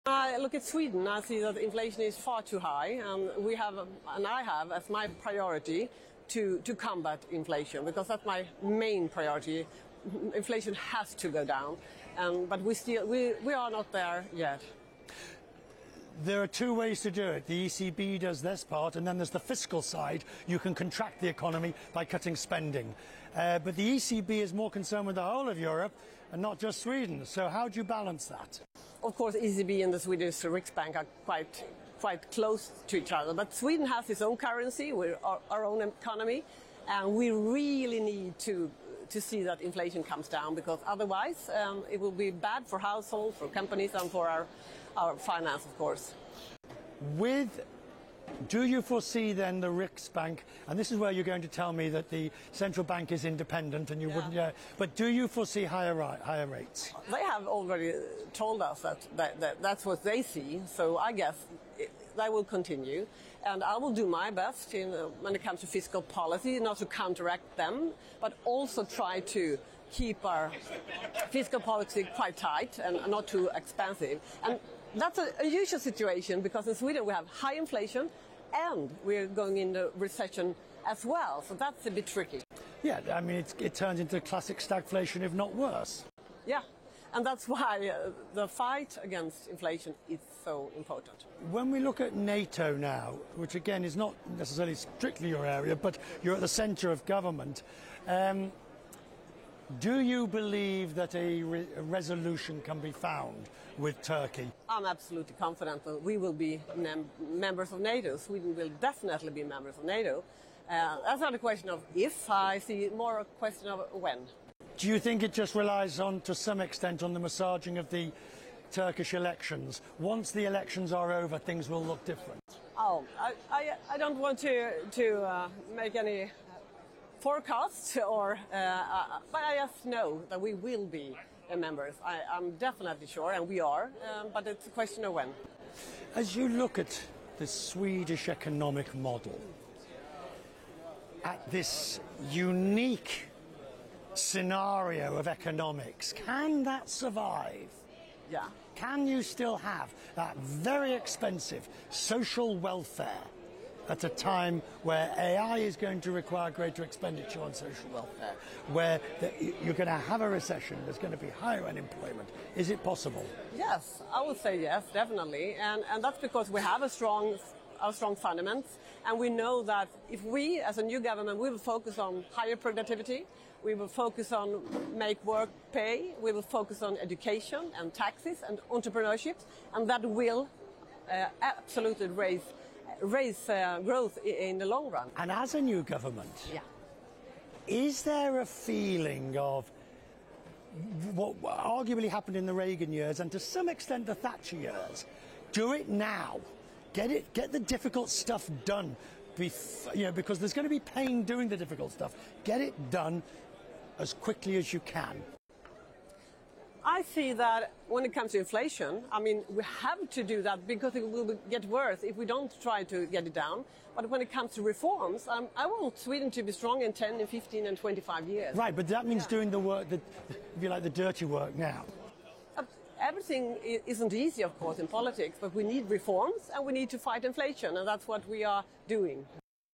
Swedish Finance Minister: We are not there yet on inflation
Swedish Finance Minister Elisabeth Svantesson discusses her country's economy and potential NATO membership.